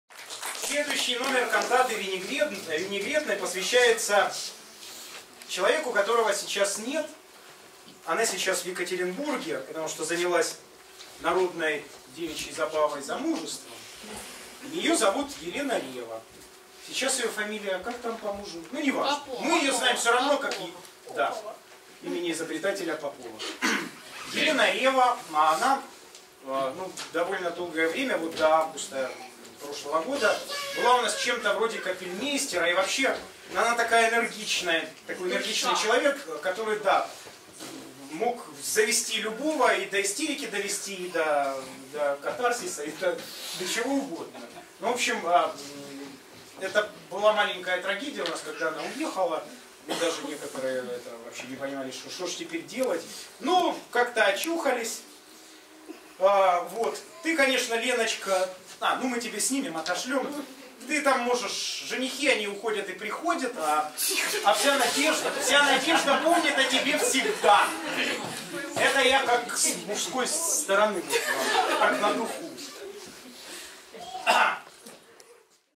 Конферанс / Посвящаем песню Елене Реве
Наша вдохновительница и воспитательница увлеклась замужеством. Относимся с пониманием, скучаем, радуемся редким встречам:)
Концерт-посиделка, созванный в честь двухлетия лаборатории аутентичного народного пения «Вся Надєжда».
Харьковская Муниципальная Галерея, 1 апреля 2015 г.

«Вся Надєжда» (в порядке размещения на сцене):
Сергей Полтавский, Артем Федяй, Игорь Авдеев, Яна Серая, Маргарита Удовиченко, Елена Кутицкая, Анастасия Яловега, Юлия Шевель, Влада Галкина

Обнимаем и говорим спасибо:
➣ Татьяне Тумасьян и Харьковской муниципальной галерее — за уютное пространство и отзывчивость.
➣ Фотографу Андрею Ярыгину (yaryg.in) — за все возможные и невозможные точки зрения.
➣ Операторам Алексу Фалькову и Евгению Титову — за фиксацию времени и пространства.
➣ Монтажеру Владе Галкиной — за КИНО.
➣ Ясе Бондарчук — за вдохновение и помощь в организации.
➣ Люсе Яковлевне, Александре Дмитриевне, Валентине Александровне и Надежде Петровне — за приятное чувство связи поколений.
➣ Ясе Бондарчук, Нестору и Горпине Федяям — за трогательное поздравление.
➣ Всем близким, родным и друзьям — за теплый прием, чуткое внимание и тонкую сонастроенность.